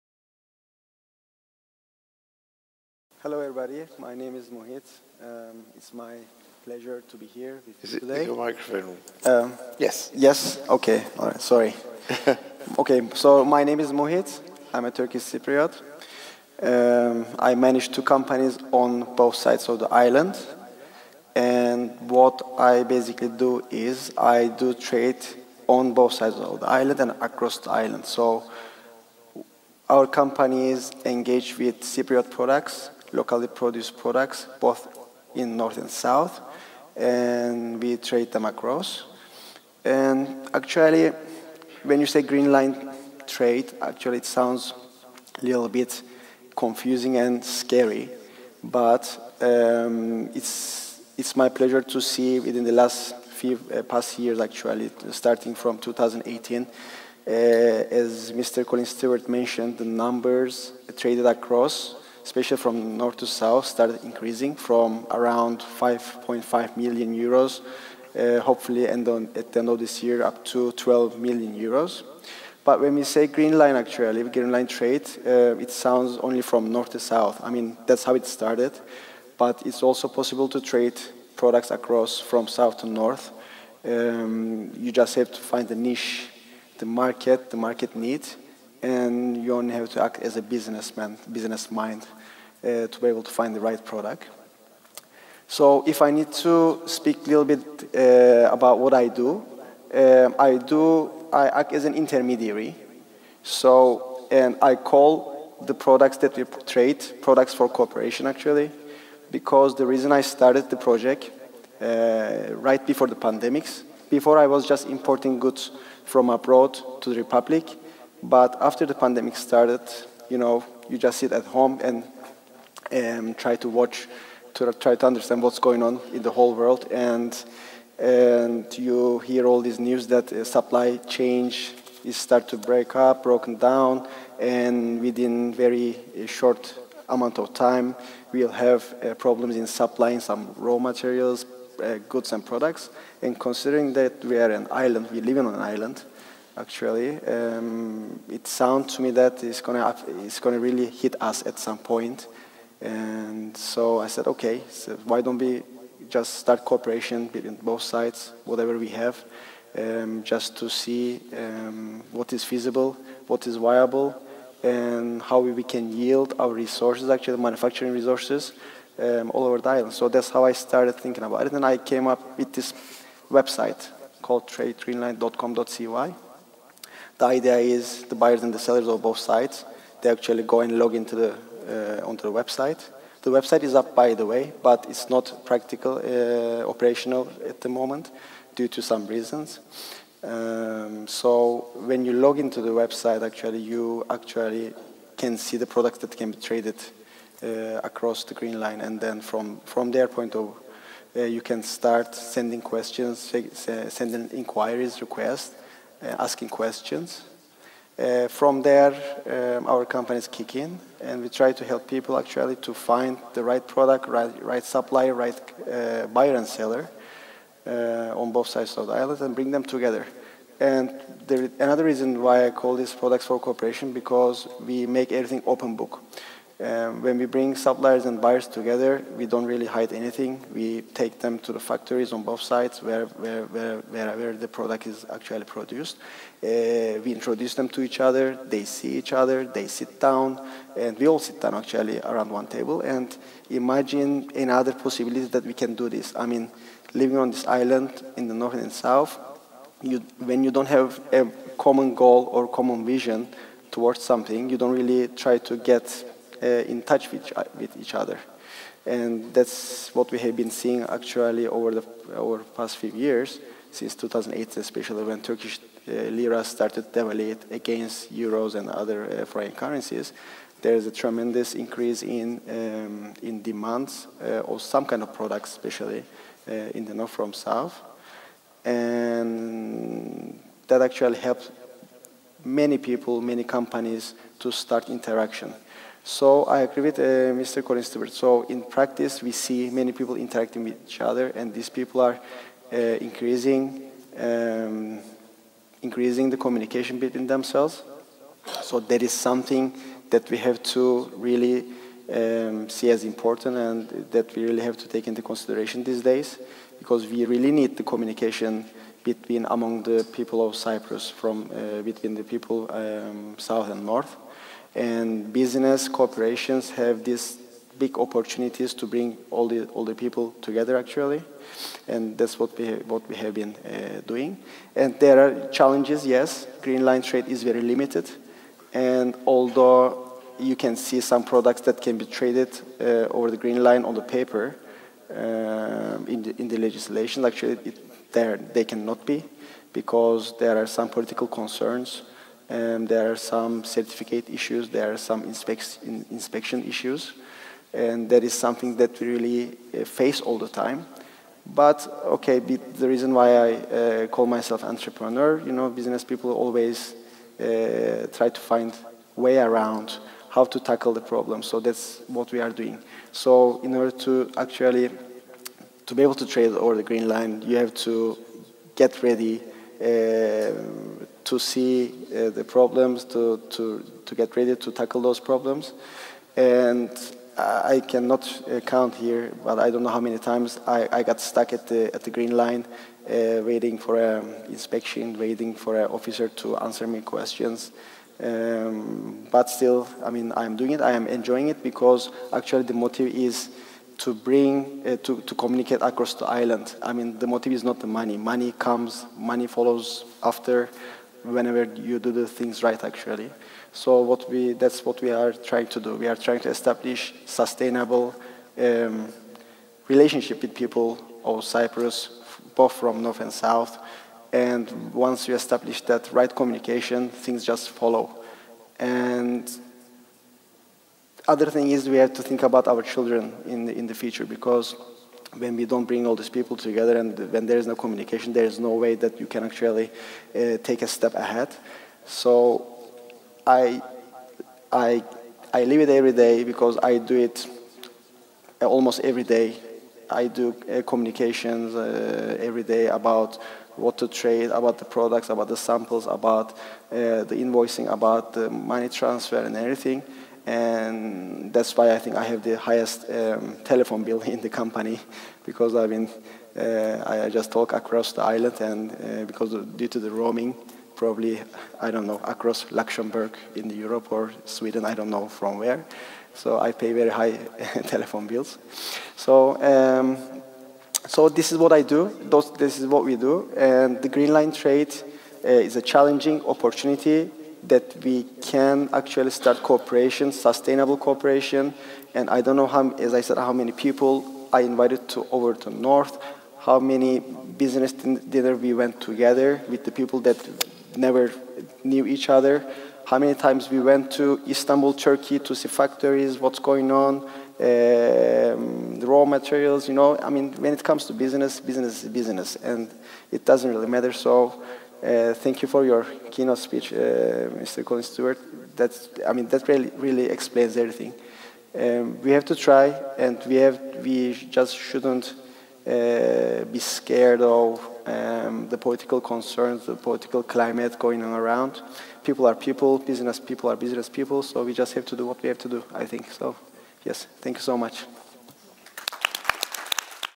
Muhit Ince on REVITALISING PEACE TALKS THROUGH INCREASED INTRA-ISLAND COMMERCE
Muhit Ince, entrepreneur, businessman at The Economist Impact's 18th Annual Economist Cyprus Summit